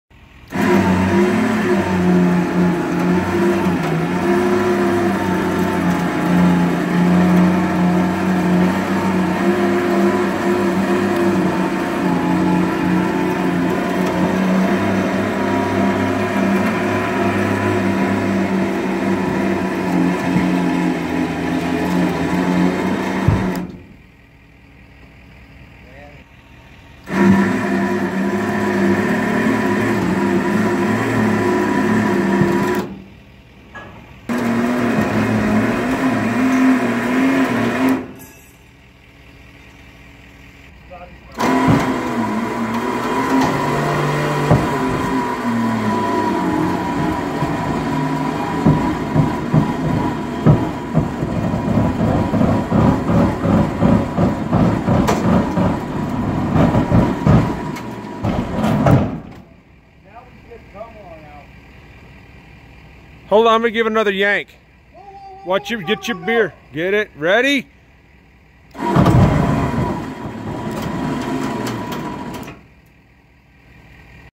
Loading a shed with a winch.
32' long shed onto a 30' trailer